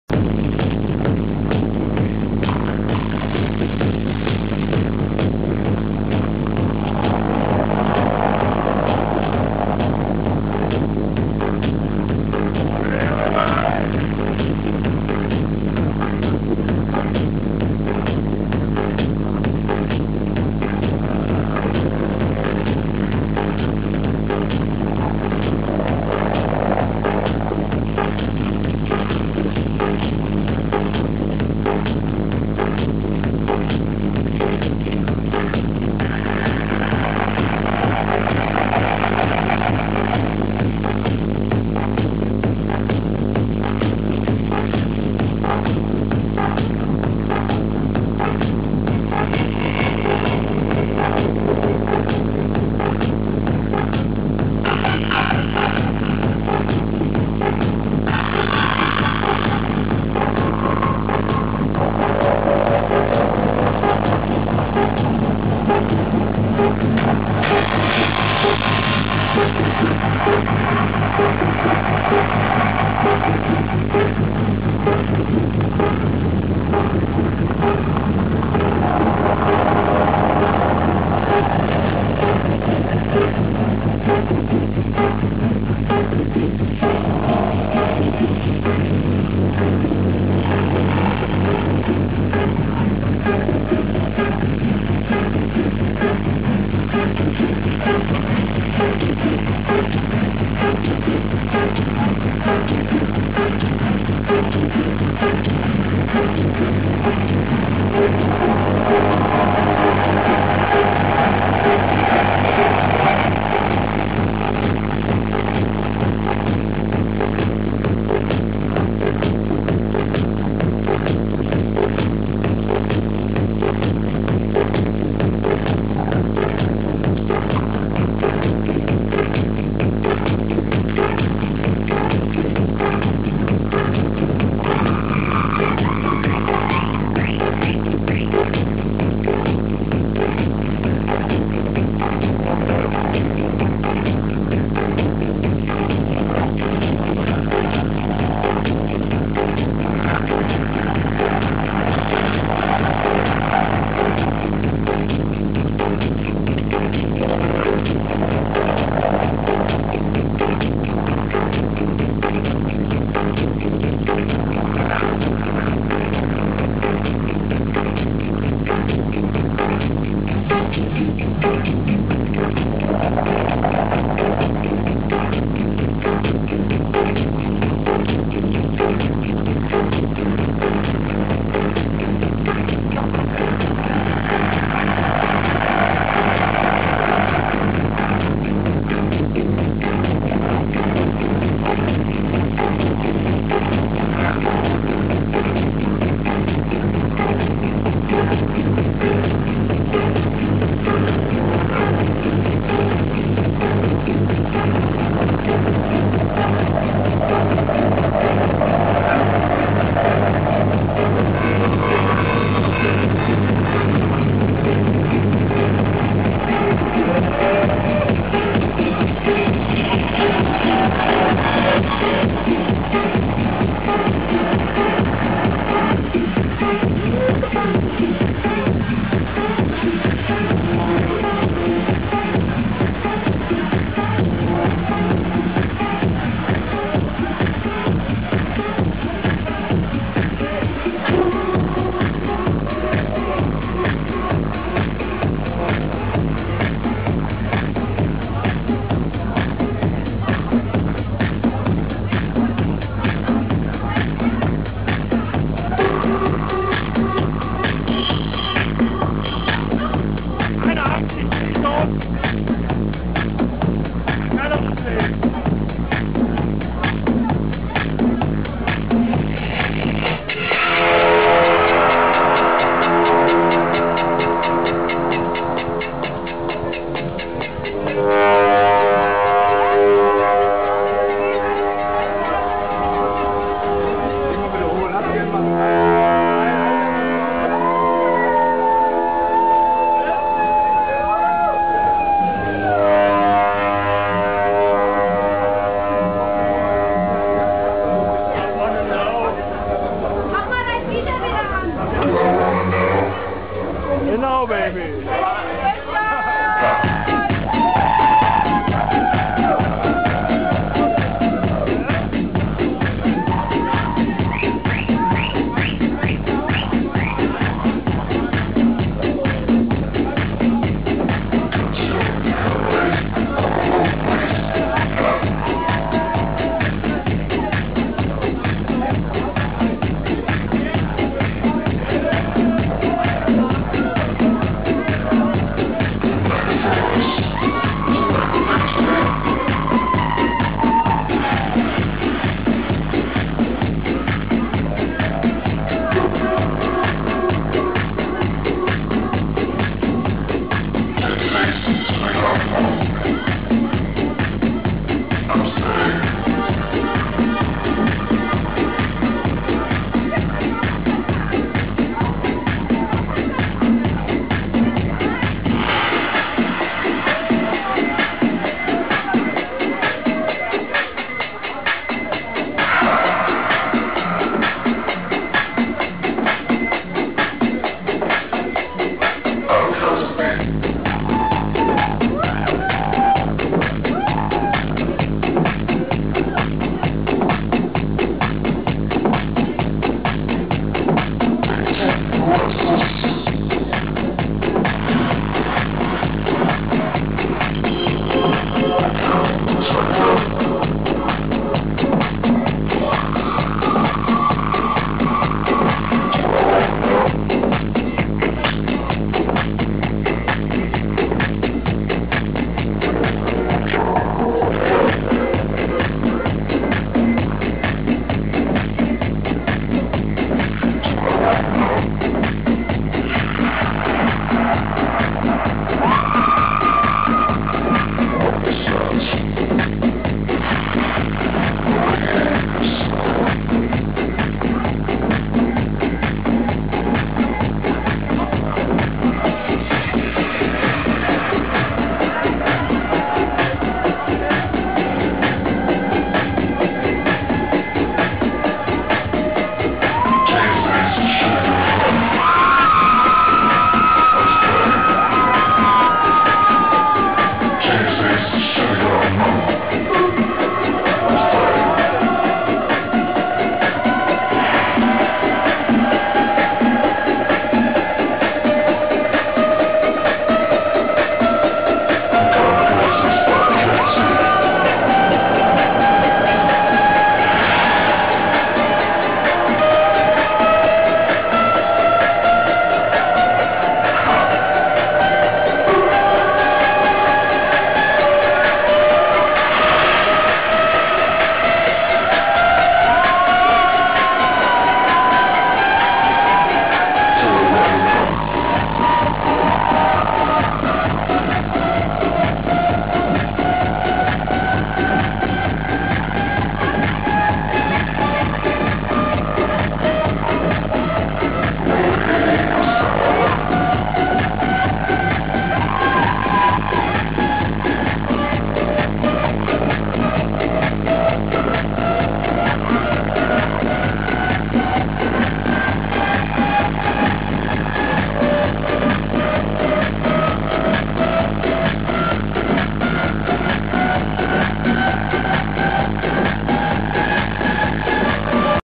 X-MAS BASH 26.12.09 Tanzhaus West Dj Stitcher Part 3
Was bleibt noch zu sagen. danke an die Crowd die so fett gerockt hat...War ein Spitzen Abend